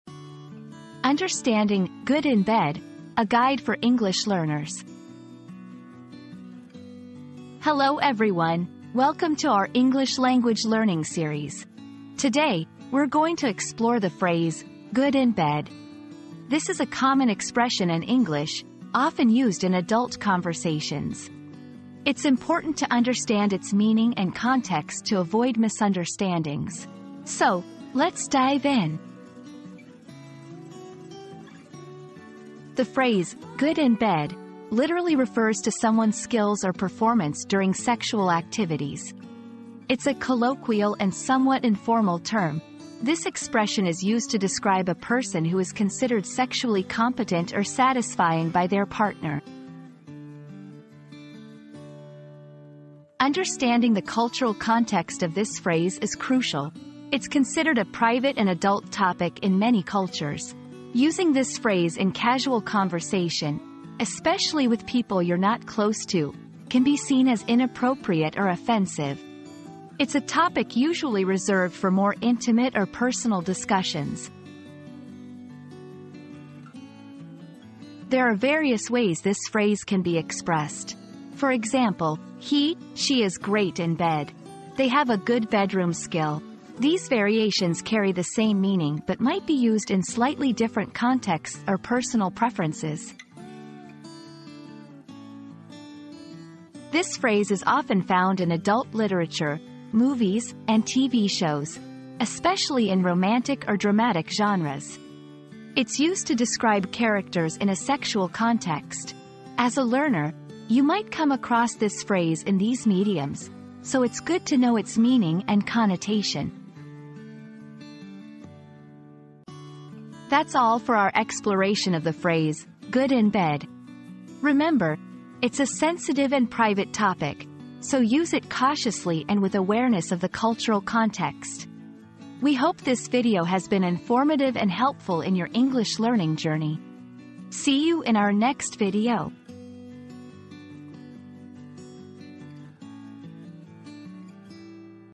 Understanding "Good in Bed": A Guide for English Learners
00:00 • Introduction - Understanding "Good in Bed": A Guide for English Learners
00:32 • The Literal Meaning
00:52 • Cultural Context and Sensitivity
01:17 • Variations of the Phrase
01:36 • Usage in Media and Literature